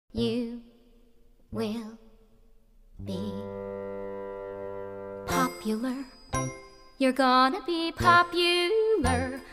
Let's make a backpack before the new season 💚
Let's make a backpack for the next anime convention!
Follow for more fan inspired designs!